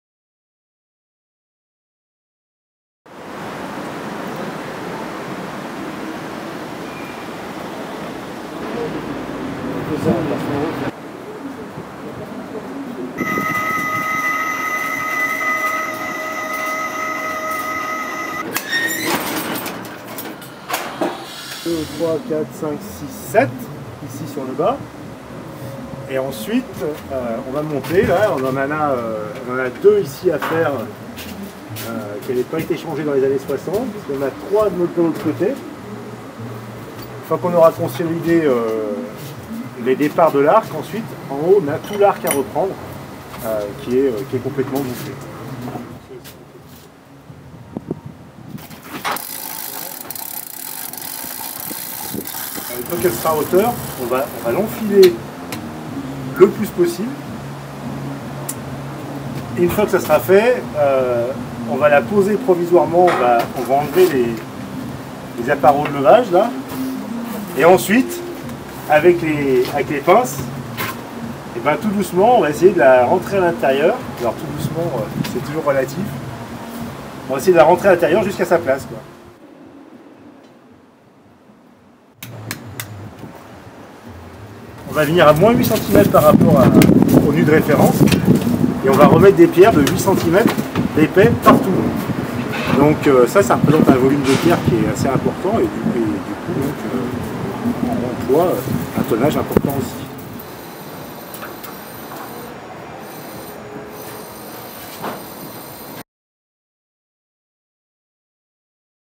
De nouvelles pierres pour la cathédrale Saint-Maurice de Vienne
Depuis plusieurs mois déjà, un échafaudage masque la tour Nord de la cathédrale Saint-Maurice de Vienne. La première étape d'un important chantier de restauration de la façade principale qui va s'étaler sur plusieurs années. Sous les protections, les artisans travaillent à redonner à la façade son lustre d'antan en changeant les pierres et en restaurant les décors.